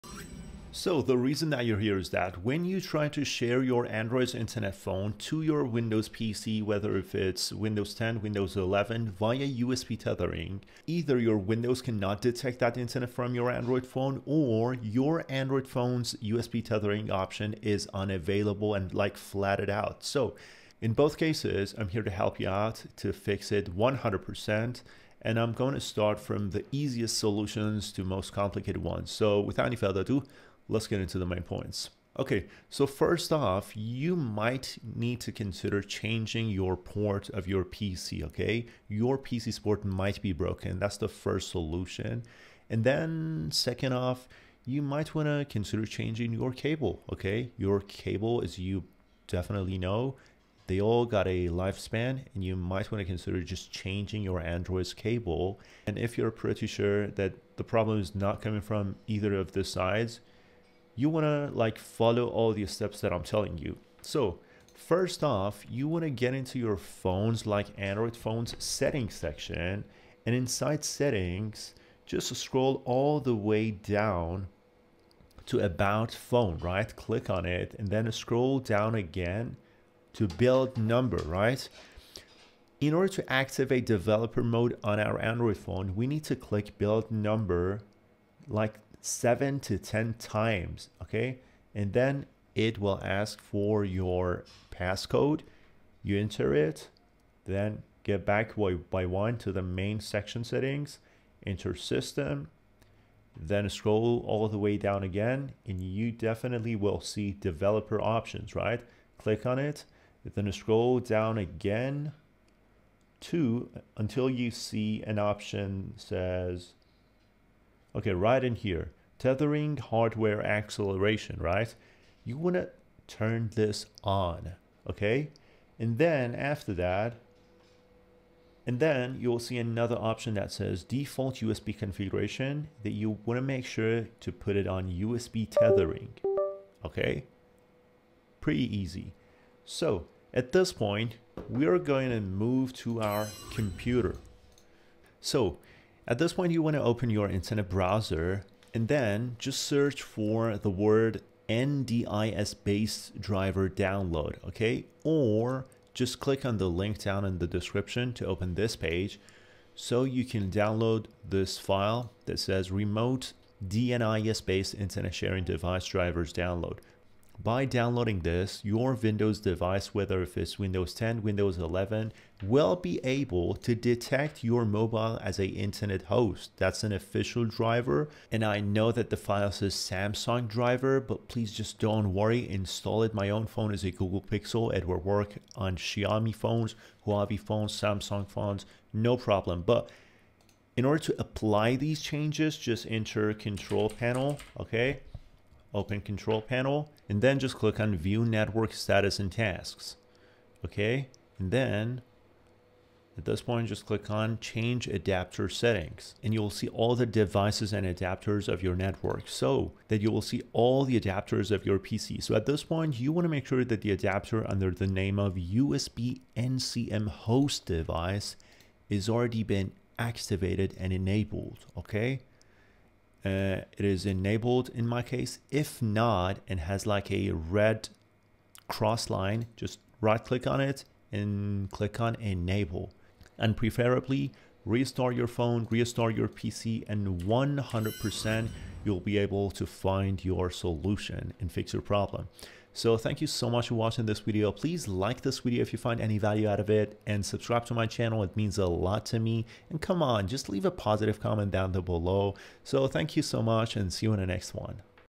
USB Tethering Android To PC Not Working!
00:00 initial Explanations
00:30 Before You Start
01:00 Main Tutorial (Follow All Steps)
03:53 Outro

Is your Android phone refusing to share its internet connection with your PC via USB tethering? Don't fret! This comprehensive video walks you through troubleshooting and fixing common USB tethering problems on  Windows 10 and 11.

Learn how to:

Diagnose and resolve connection issues
Configure Android settings for optimal tethering
Update drivers on your PC for smooth connectivity
Troubleshoot specific errors like "Remote NDIS based Internet Sharing Device" not working
Optimize your connection speed for a seamless browsing experience

This video is perfect for:

Students and professionals needing reliable internet access on the go
Travelers relying on their phone's data plan